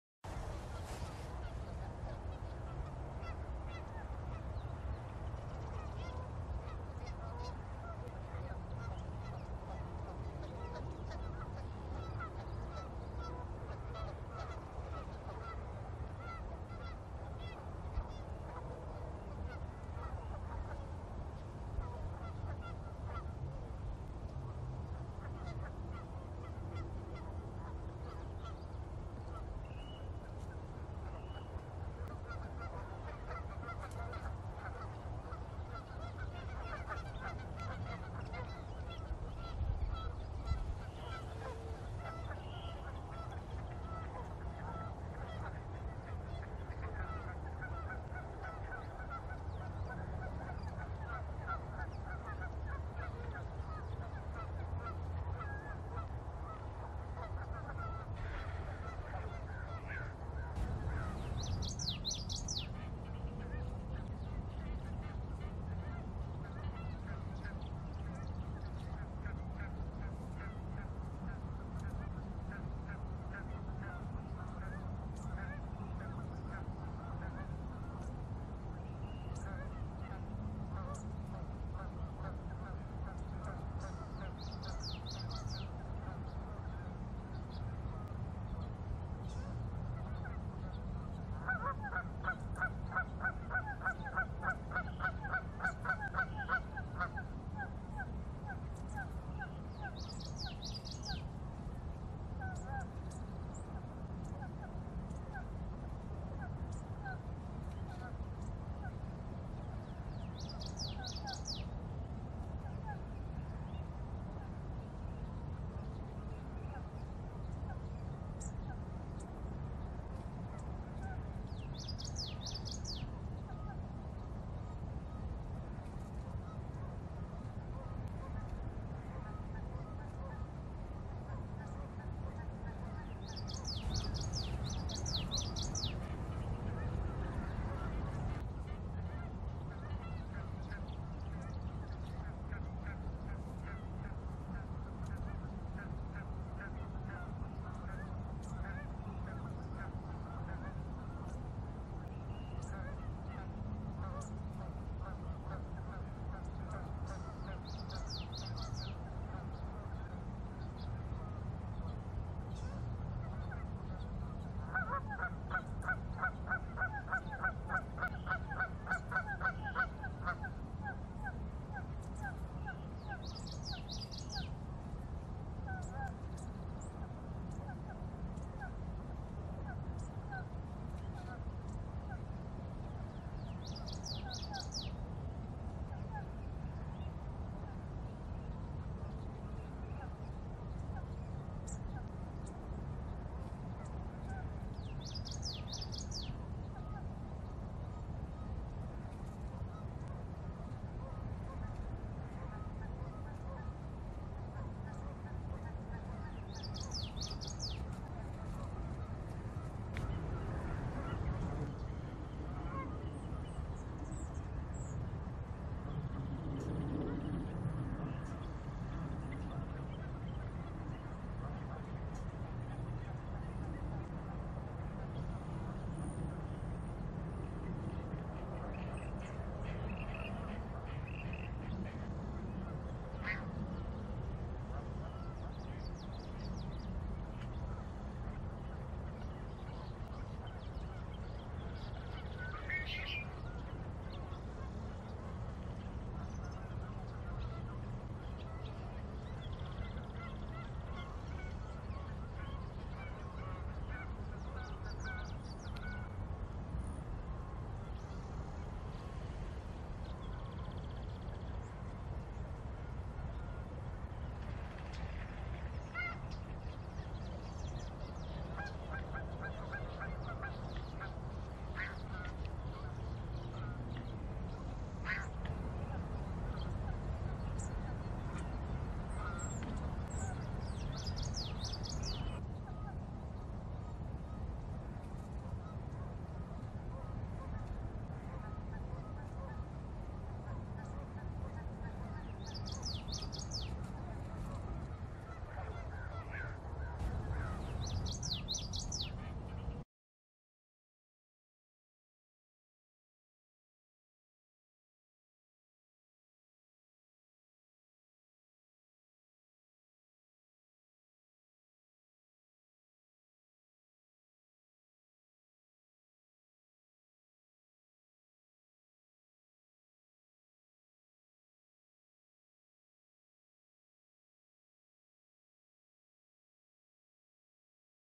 Amazing Great Blue Heron in epic battle with large gopher
Spoiler alert, the heron wins.  Never bring gopher incisors to a dagger fight.